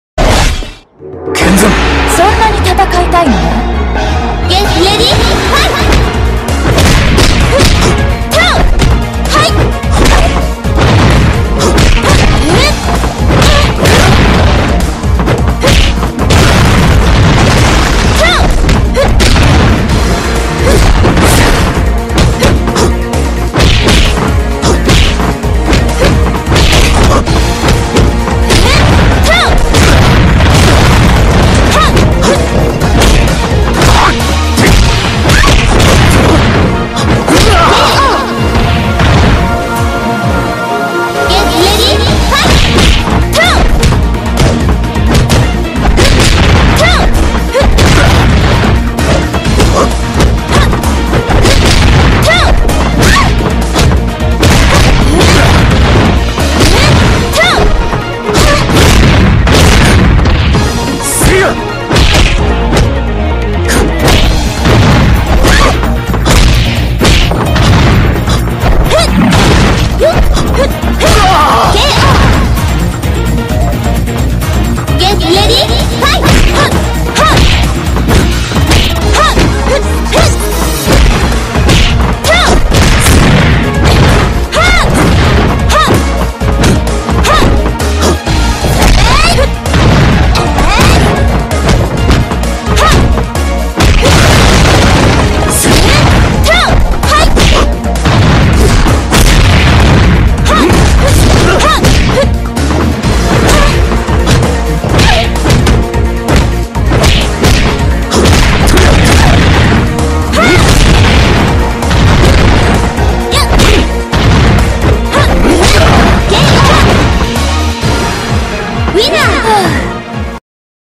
FatalxInnocence (LIS) vs Traviolly (HAY) 3
Traviolly pulls his Hayate back out whilst Lisa steals Lei-Fangs black dress? =S

I'll try get some footage of the special set 2 pack tomorrow x__x for now I must sleep. Hope you enjoyed these set of videos! =D If you wanna see anything else let me know.